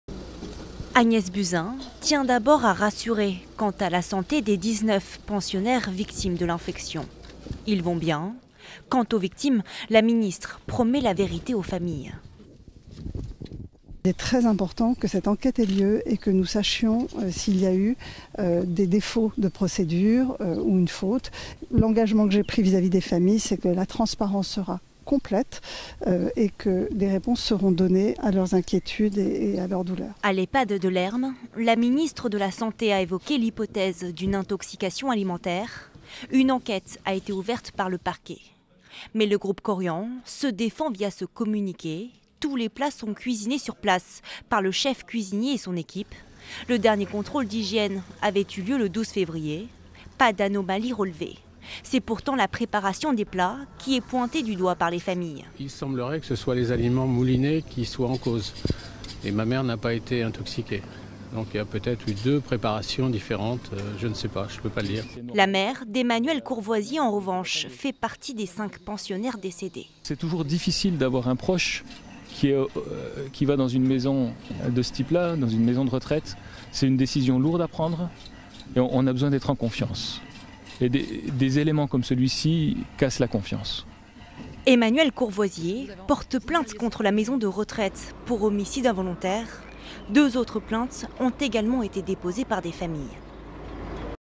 Ehpad de Lherm : trois plaintes déposées, Agnès Buzyn se rend sur place
Trois familles ont porté plainte après le décès de cinq personnes âgées des suites d'une possible intoxication alimentaire, dans la nuit de dimanche à lundi. La ministre de la Santé Agnès Buzyn s'est rendue à la maison de retraite de Lherm.